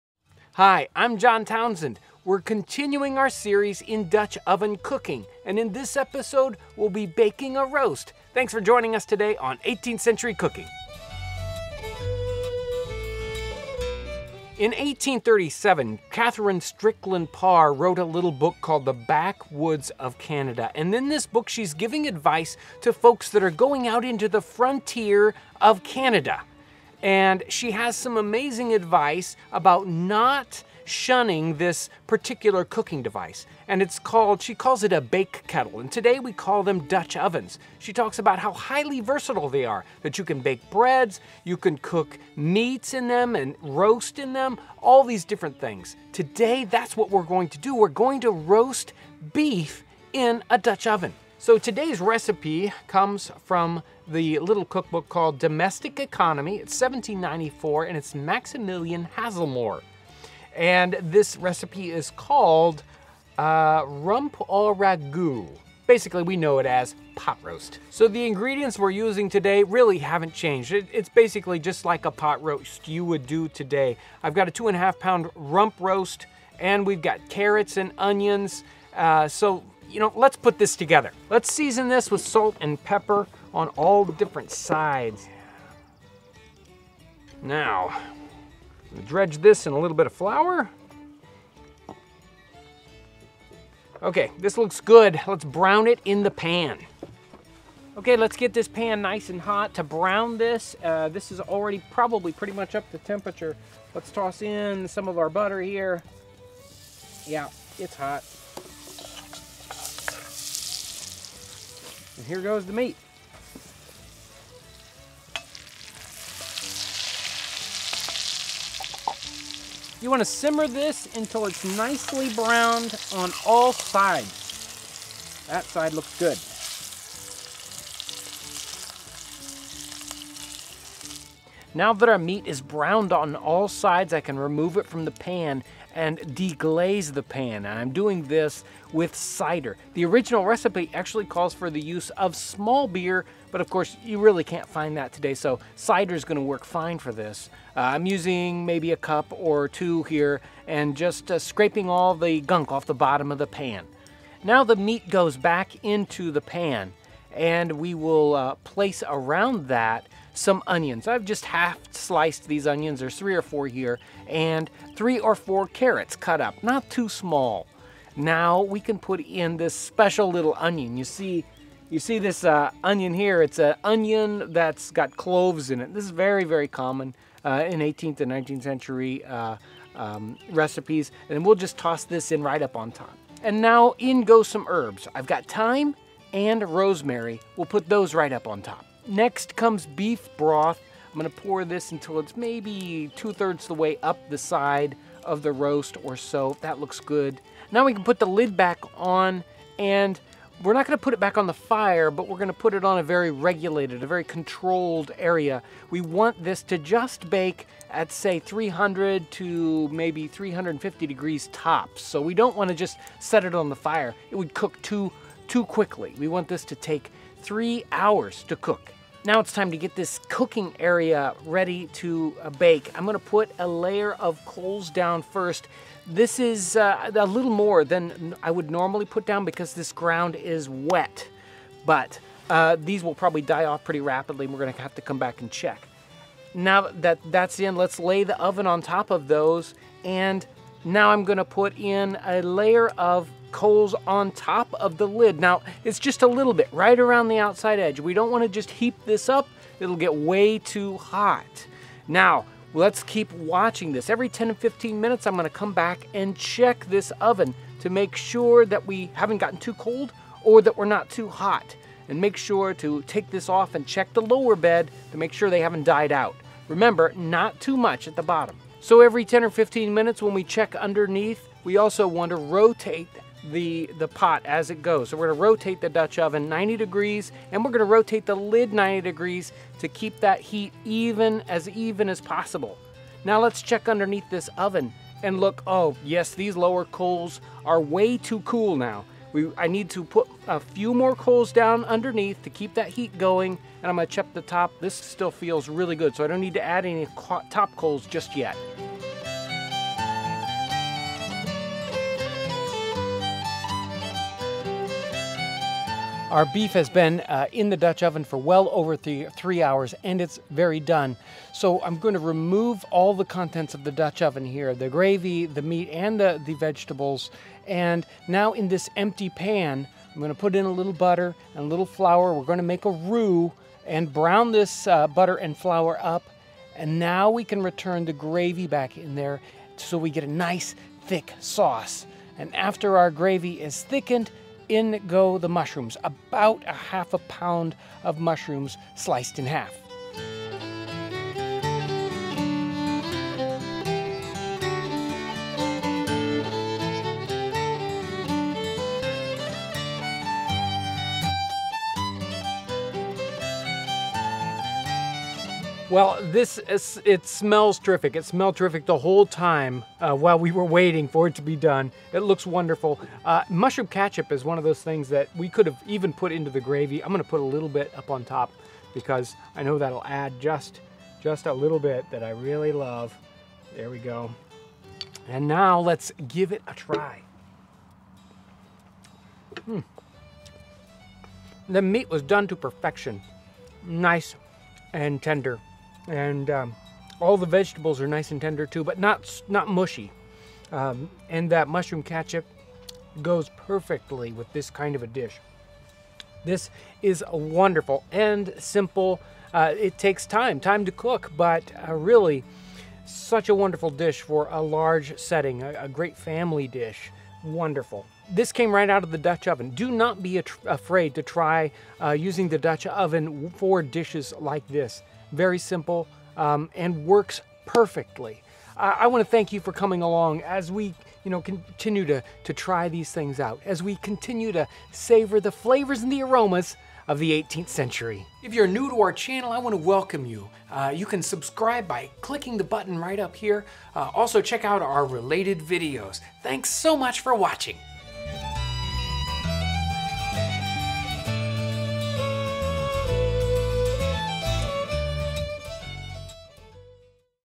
Delicious 1794 Roast Beef! - Dutch Oven Cooking
New Instagram - @18thcenturycooking
This is the second video in a short series on baking in a "Bake Kettle" or Dutch Oven. Today we prepare an absolutely wonderful roast beef recipe from the "Domestic Economy" cookbook written in 1794 by Maximilian Hazelmore.

The Dutch oven was perfectly suited for use on the frontier. One can fry in it, make stews and soups with it, as well as bake in it. While the first two cooking methods are fairly easy, baking with a Dutch oven can be a little intimidating. With a few hints and a little experimentation and practice, baking in this 18th-century pot can be easy and rewarding.

More great information!